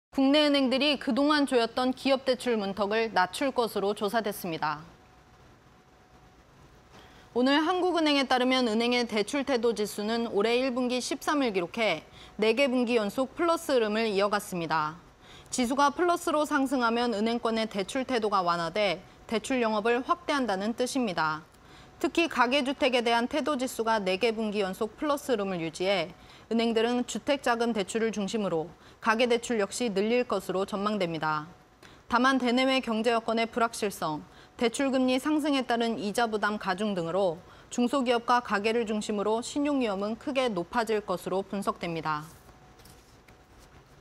은행, 1분기 대출문턱 낮춘다... 이자부담에 신용위험 사상 최고 (20230118)
은행, 1분기 대출문턱 낮춘다... 이자부담에 신용위험 사상 최고